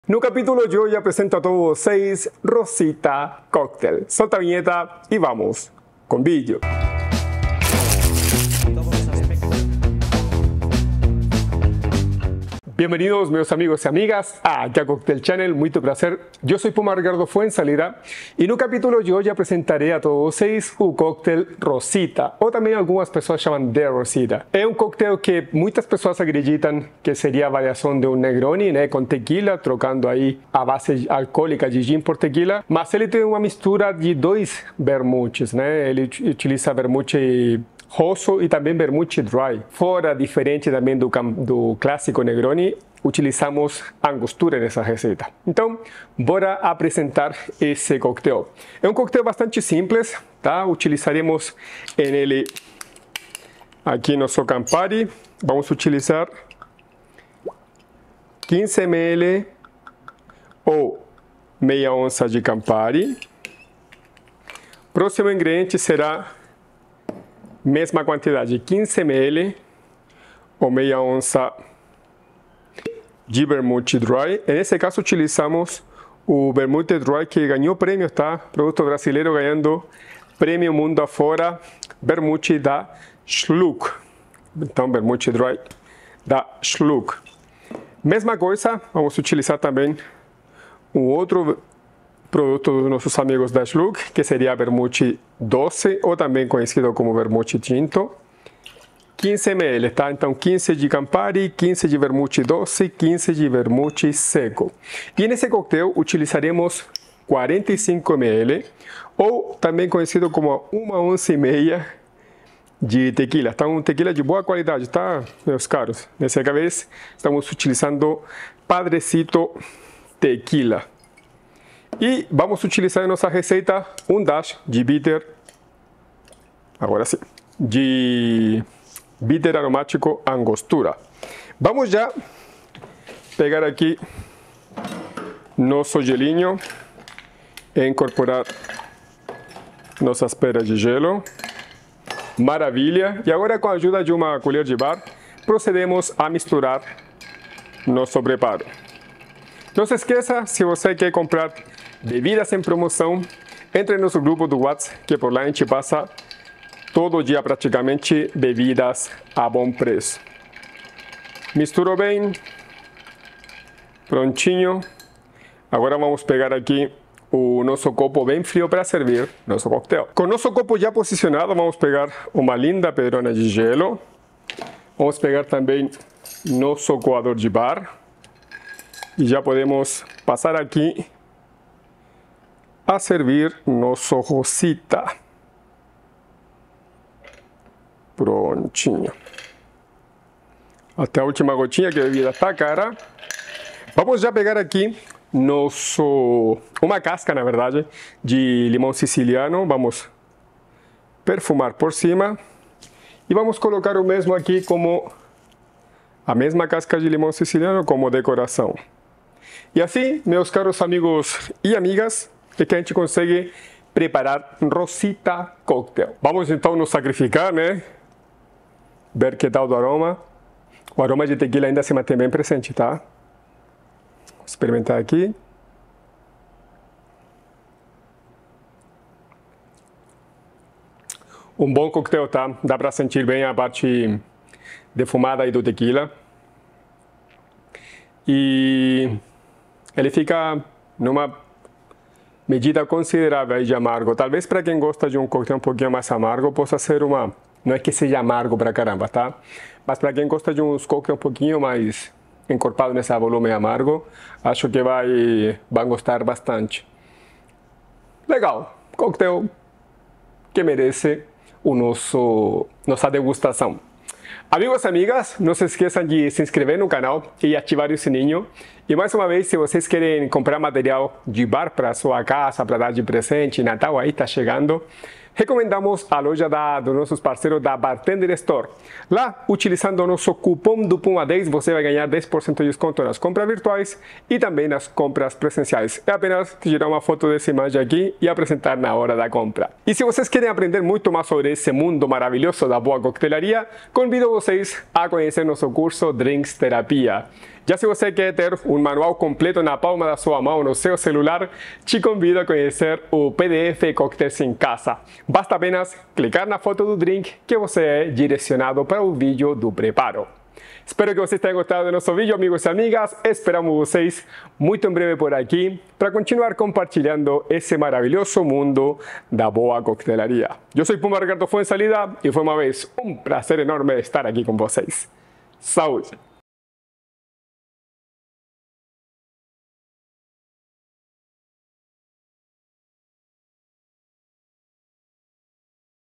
COMO FAZER ROSITA RECEITA DE NEGRONI COM TEQUILA 🥃🇲🇽
The Rosita • Coquetel com Tequila  🥃🇲🇽
45ml Tequila Blanco
15ml de Campari
15ml de Vermouth Doce - Tinto
15ml de Vermouth Dry - Seco
1 Dash de Bitter Aromático Angostura
Perfumar e Decorar com casca de Limão Siciliano

Beba Menos, Beba Melhor.

Curso Completo de Coquetelaria DRINKSTERAPIA e faça em casa Cocktails igual um profissional

TikTic @Puma.Cocktail
Facebook.com/CocktailChannel